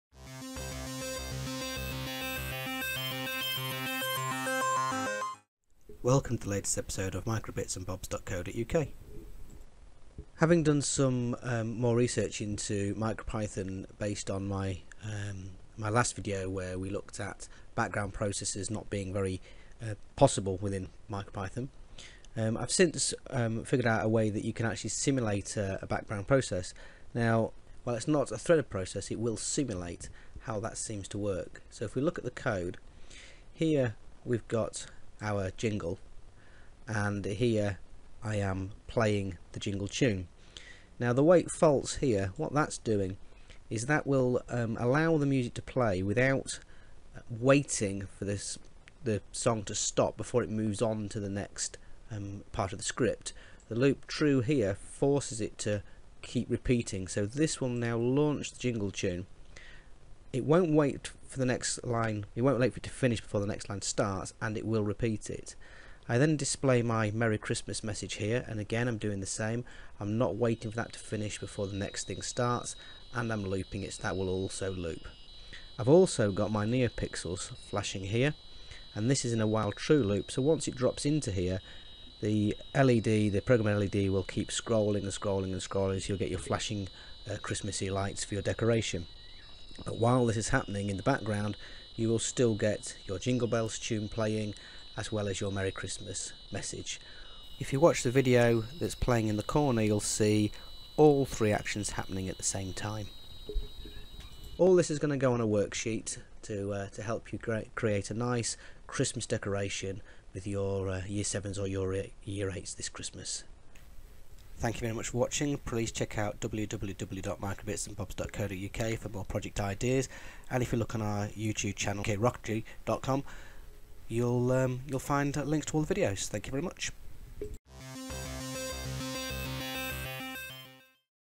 Background Process in MicroPython and Micro:bits
Project 31 - Christmas worksheet.  Making 3 things happen at once using simulated background processing using MicroPython